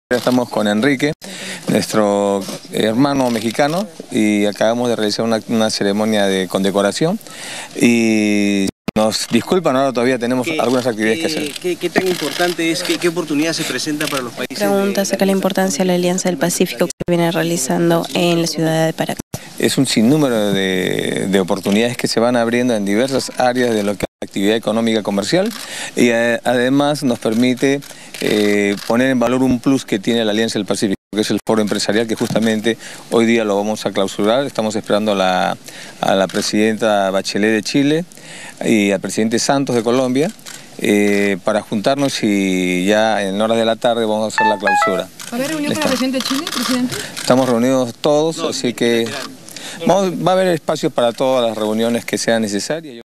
Presidente OllantaHumala en Paracas
La Alianza del Pacífico genera un sin número de oportunidades, resalta el presidente Ollanta Humala, desde Paracas.